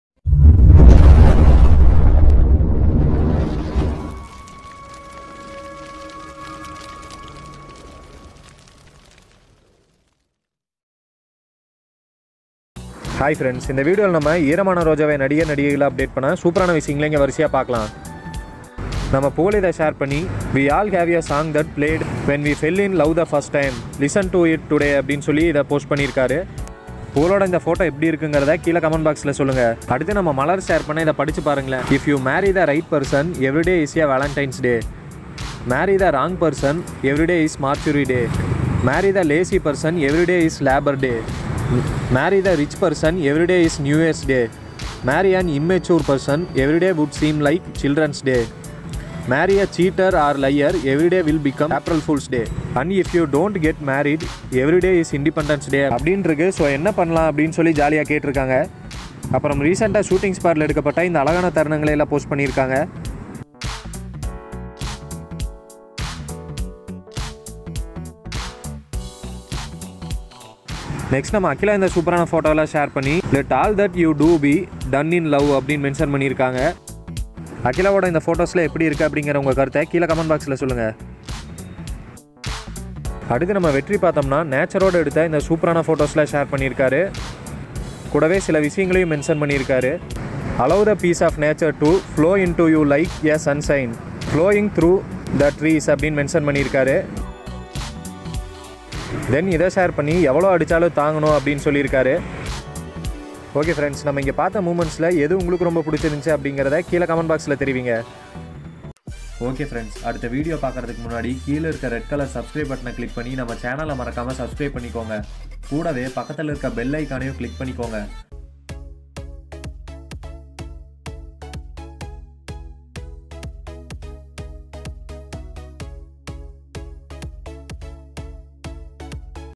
eeramana rojave serial vetri malar 😍 pugal akila latest video! | marriage
கல்யாணம் பற்றி மலர் என்ன சொல்லுறாங்கனு பாருங்க!

ஈரமான ரோஜாவே வெற்றி மலர் புகழ் அகிலா வீடியோ!

Eeramana rojave is a famous tamil serial which is telecasting in vijay tv. In this video we are going to see about the interesting updates of eeramana rojave serial vetri, malar, akila, and pugal and marriage opinion of malar.